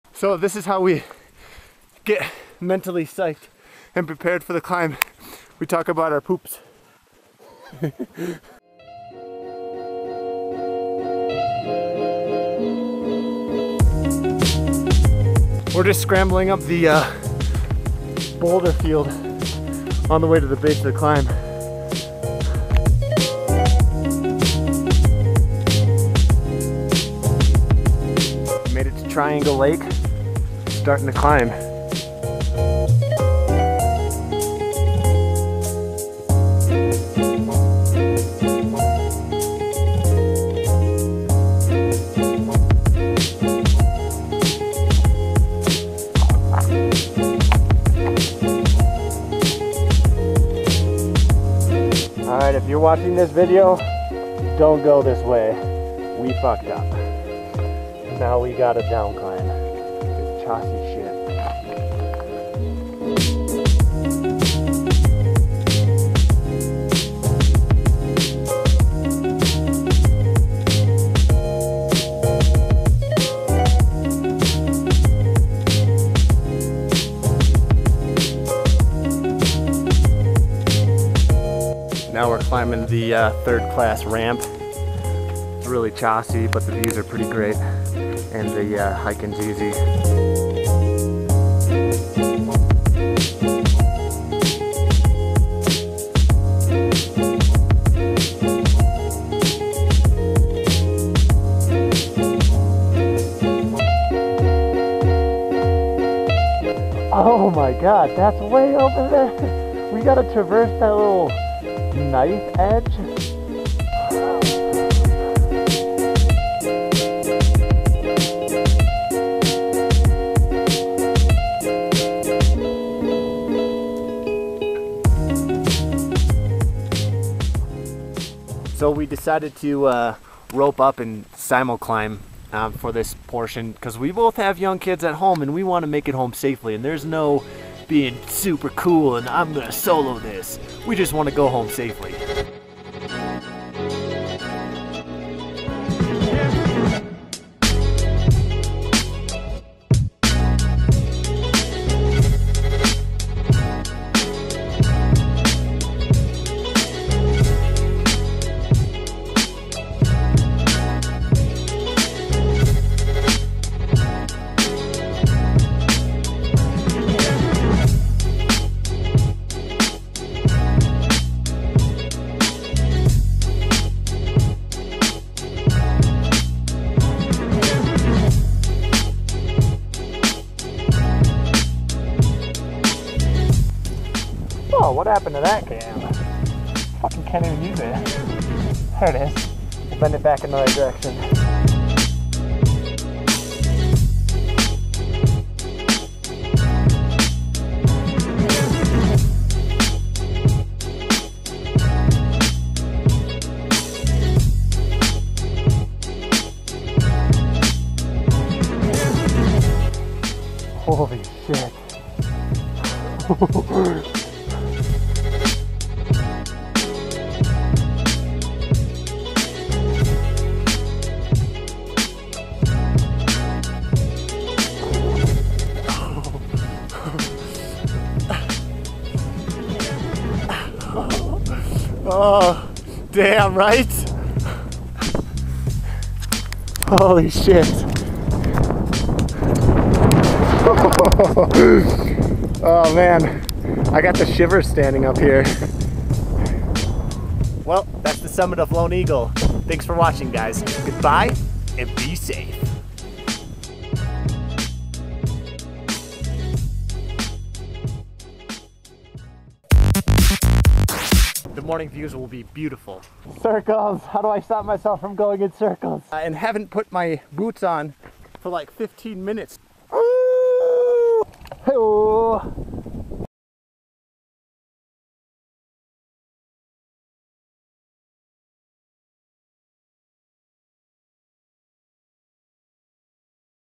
ALPINE CLIMBING ADVENTURE ----- Lone Eagle Peak, CO ----- ( trad climbing )
Six days in the Indian Peaks Wilderness Area gave Jake and I plenty of time to enjoy the pristine alpine lake, eat lots of processed junk, listen to the rain trickle on our tent, and of course summit two mountains.

The video highlights our climb up the "fourth class" scramble called Solo Flight which is used to descend Lone Eagle Peak from the classic North Face route (5.7). Our attempt at the North Face was riddled with wet rock so we turned around and opted for the much easier route to the summit. We decided to rope up for a portion of Solo Flight because the chimney (which you have to descend to the summit?) was full of loose rocks and choss. The route finding was a little confusing but that just made the adventure more exciting and there were plenty of cairns to boost our confidence when we thought we got off route.

~Logan
TREEfool.com